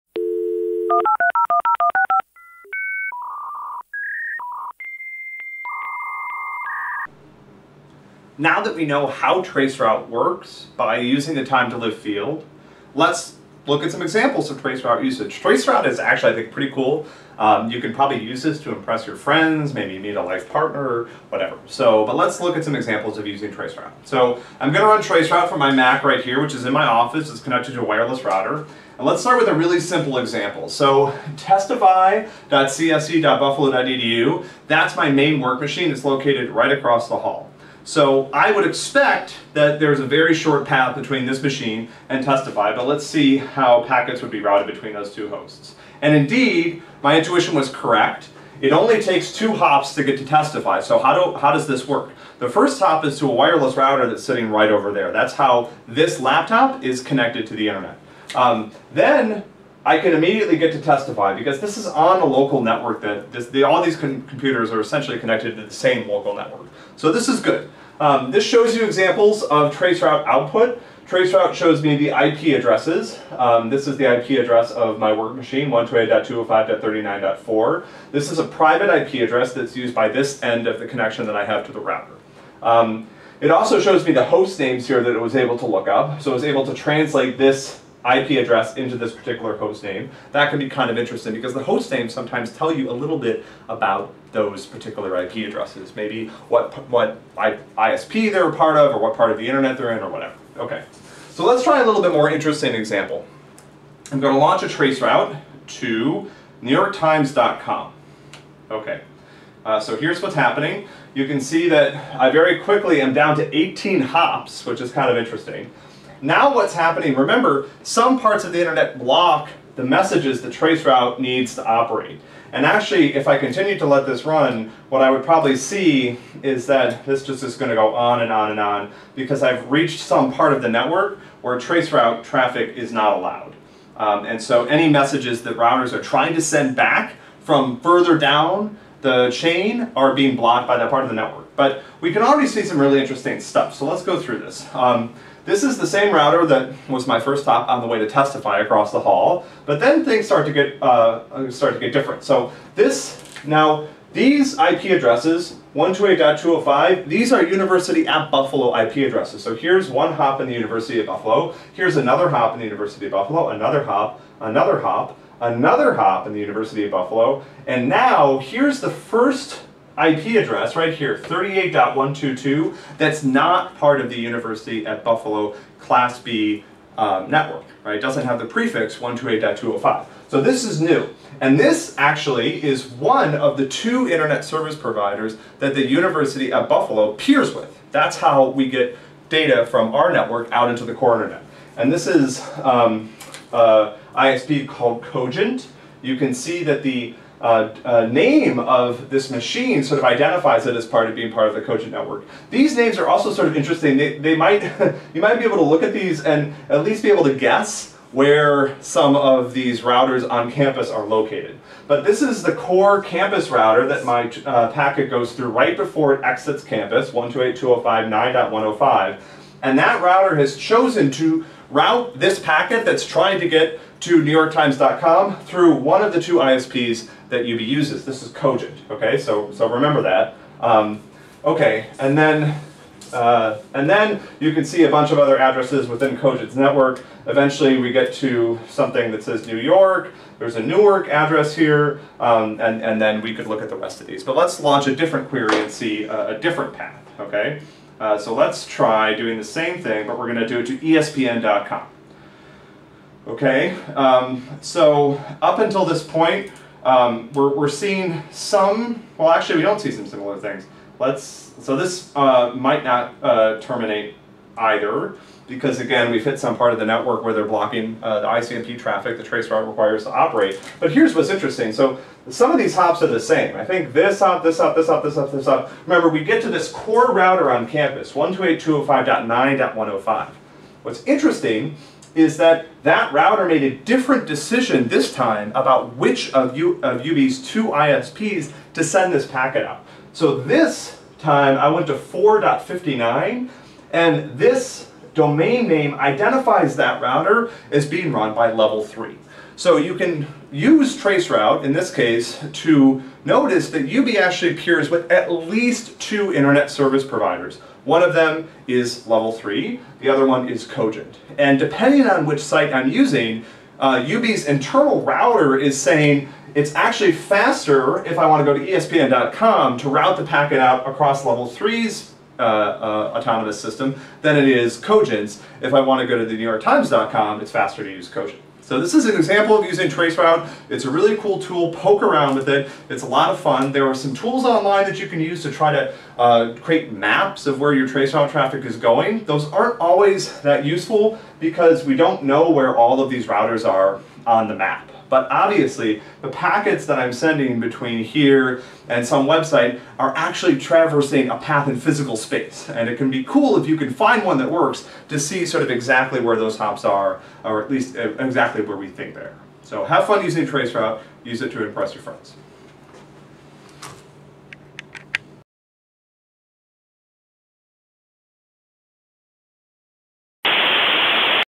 Examples of traceroute usage.
Let’s have some fun with traceroute! We’ll inspect a few routes to different internet destinations and try to see if we can map them into physical space.